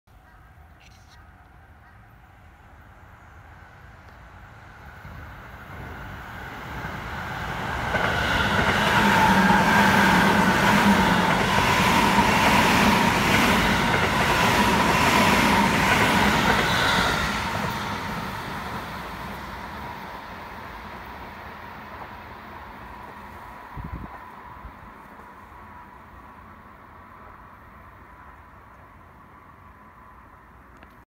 【走行中】西武新宿線新所沢付近を走行する6000系急行本川越行き(2615列車)
令和3(2021)年1月31日撮影

6000系は6001編成